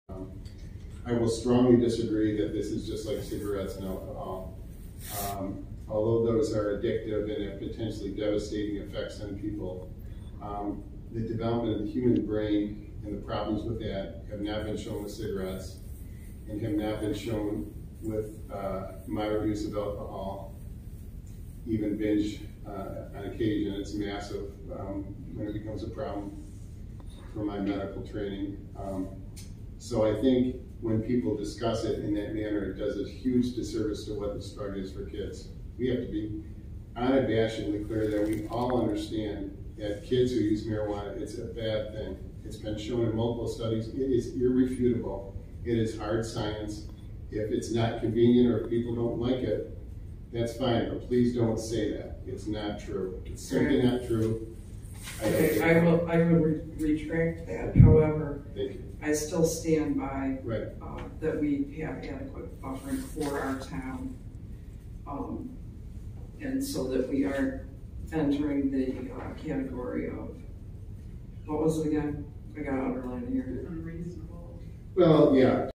Planning Commission special meeting, 10-6-22, Petoskey
Dr. Ted Pall discusses marijuana vs cigarettes/alcohol